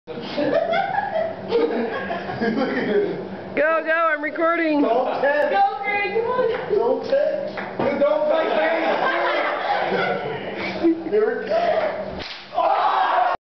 Death Ping Pong 1
Mark getting tagged in the back by Craig.